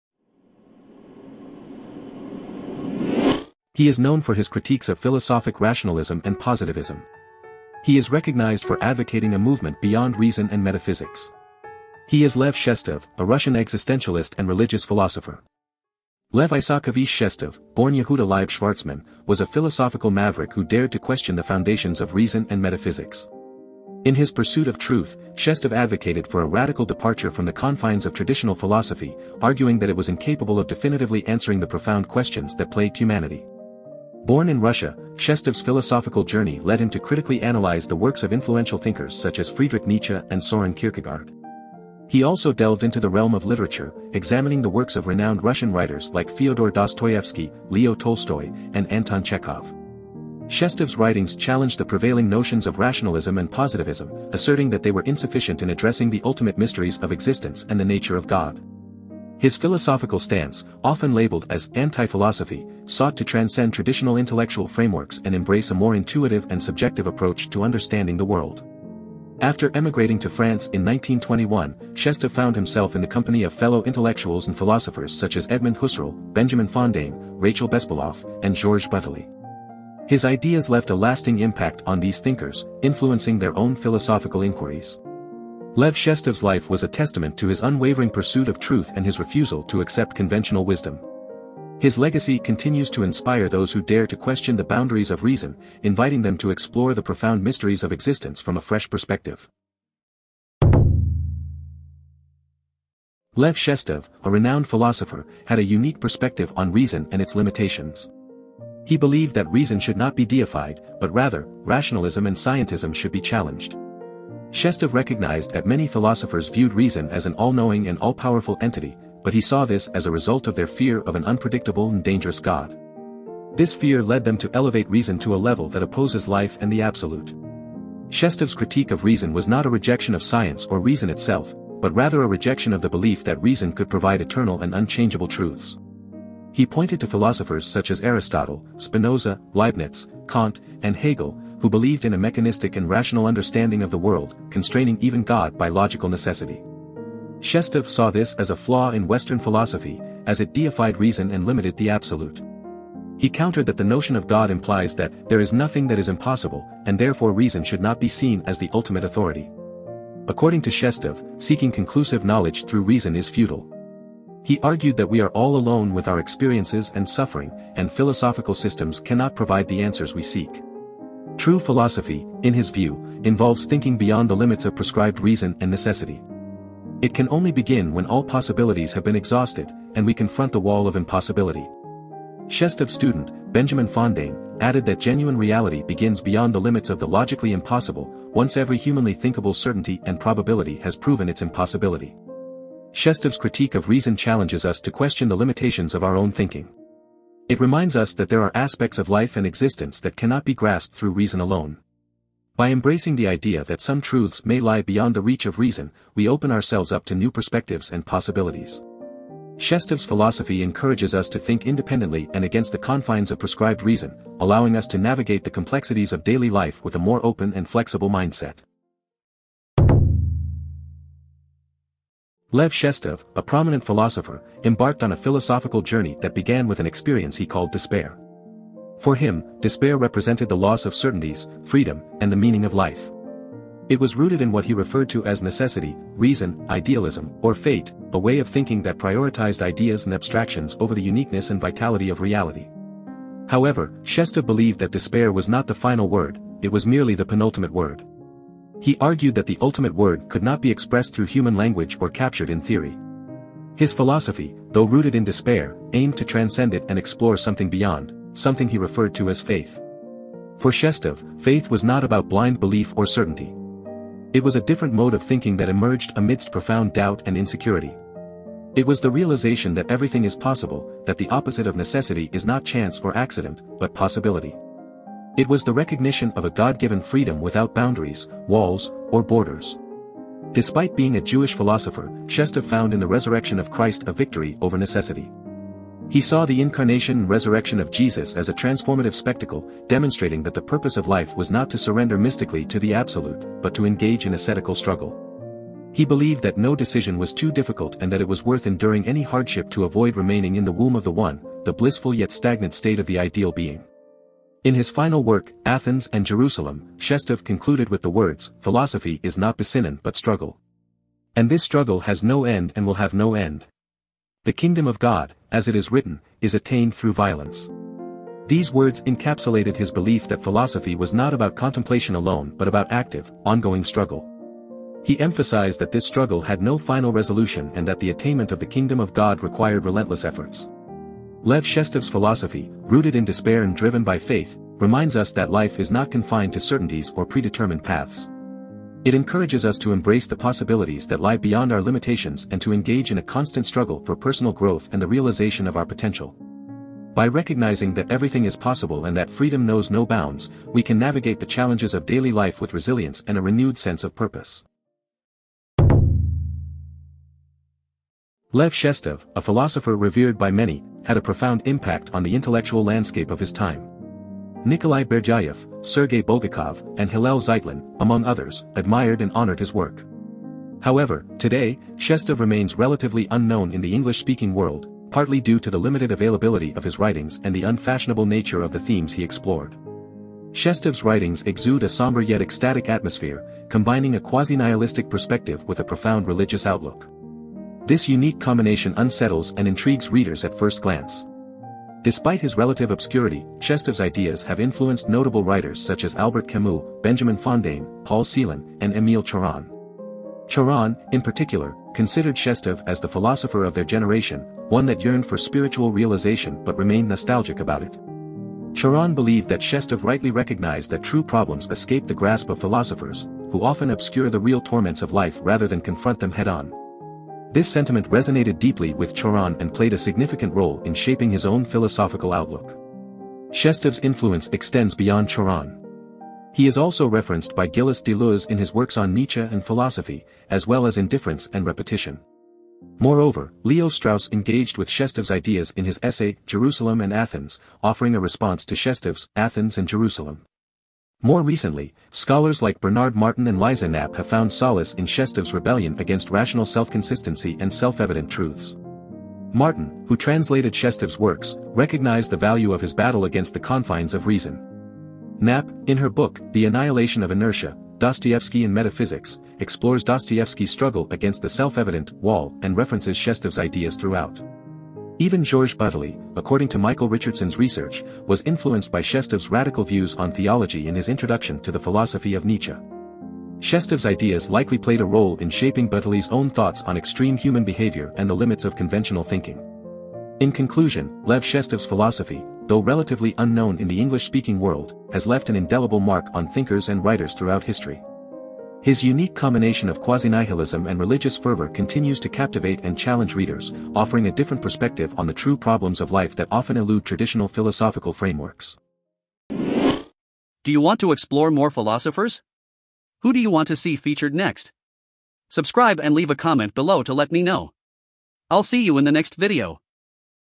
Lev Shestov: Challenging Reason and Metaphysics｜Philosopher Biography
Lev Isaakovich Shestov, born Yehuda Leib Shvartsman, was a philosophical maverick who dared to question ……

8b05